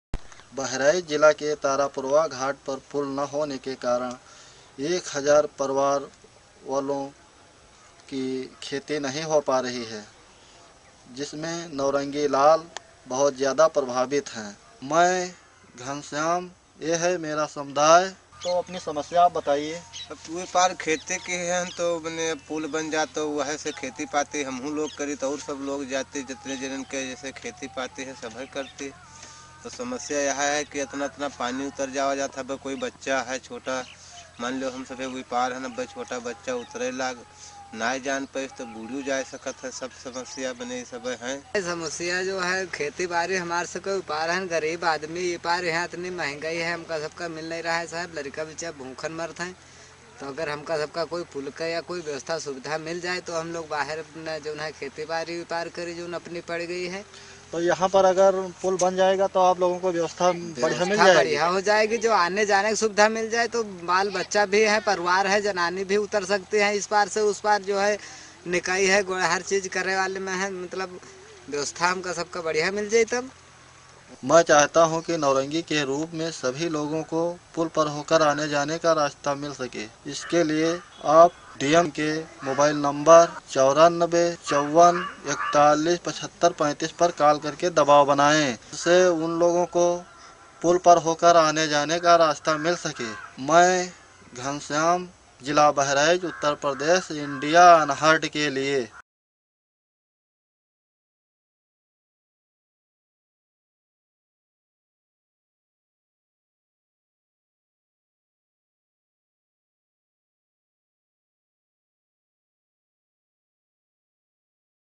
Need Bridge in Tarapurwa, Uttar Pradesh - Video Volunteer Ghanshyam Reports
This video was made by Video Volunteers Community Correspondent Ghanshyam